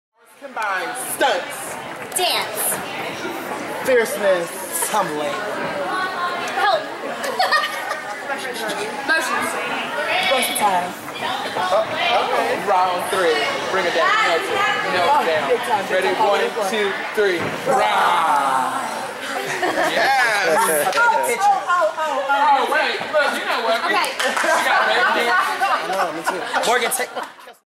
Rah! Spirit Coaches Circle
Rah! Spirit, Chattanooga, TN hosted their summer camp. Keep up the great work.